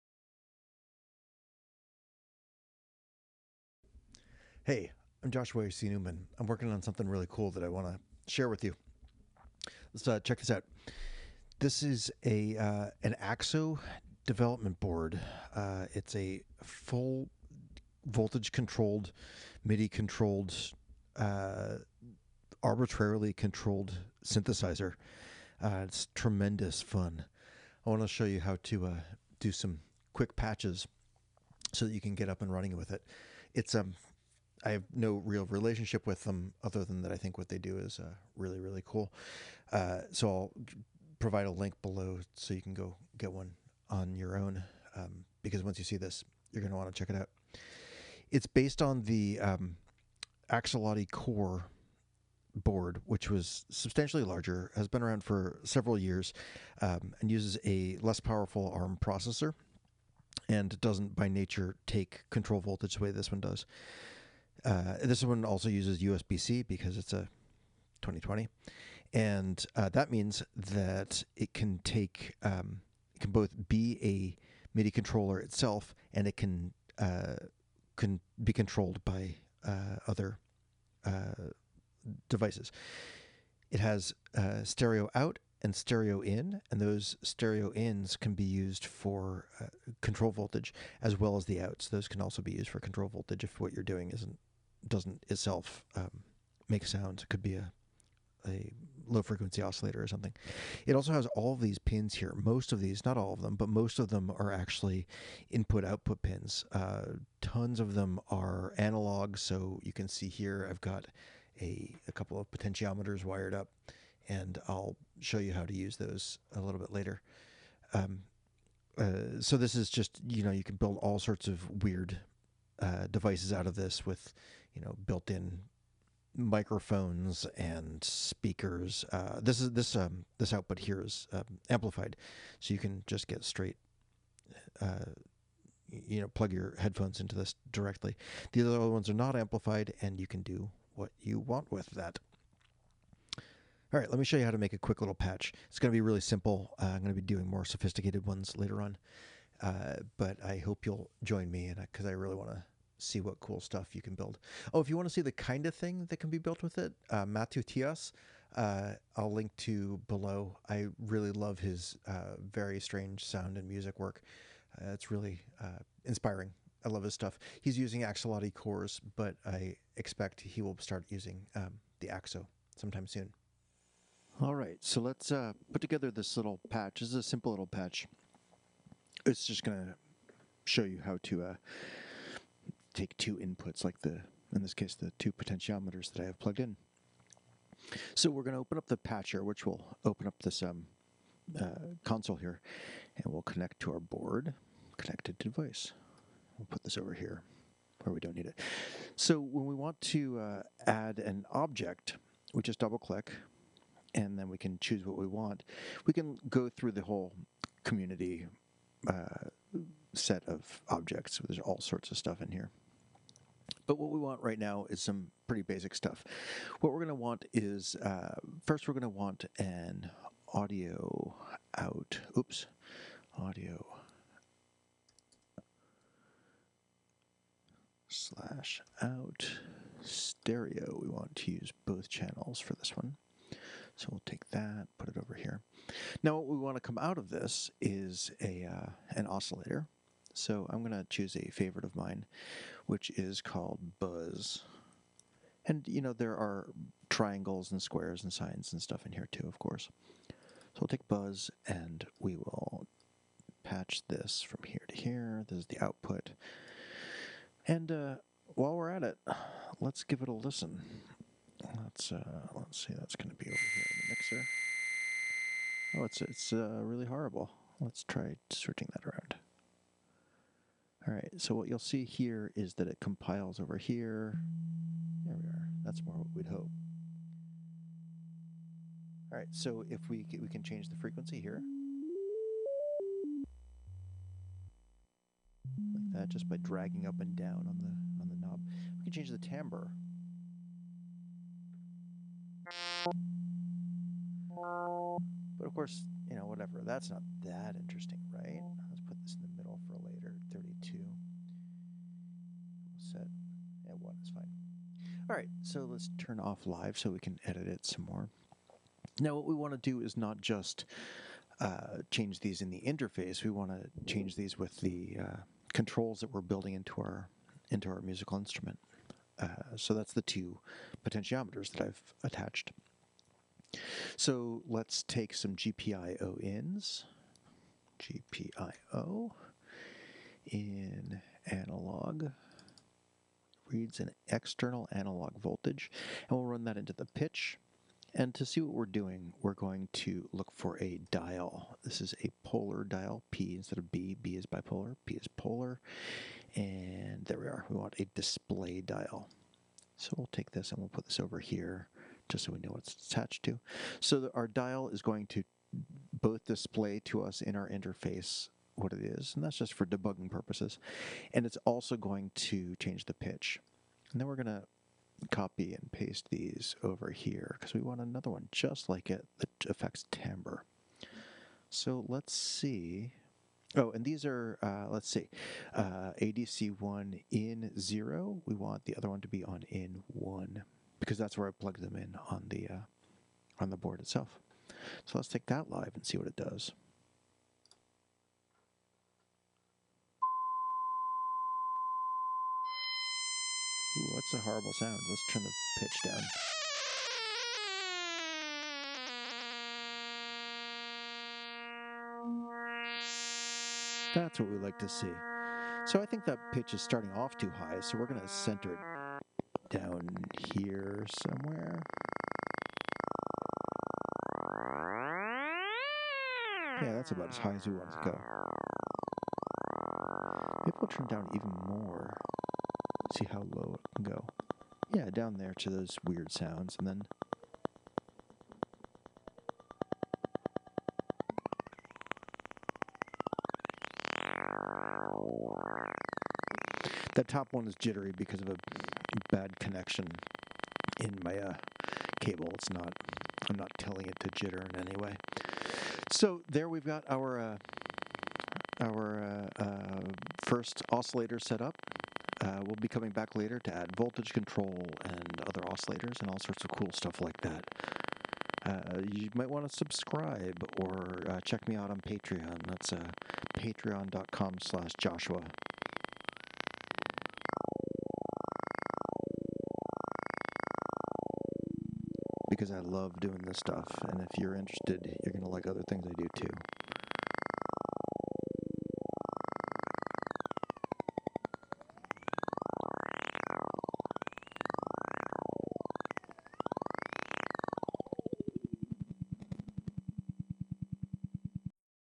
Akso Tutorial 1
I'm really excited to have an Akso synthesizer development board to mess around with! I want you to mess around with it, too! It's like $90. There's a Discord and a forum you can link to from there!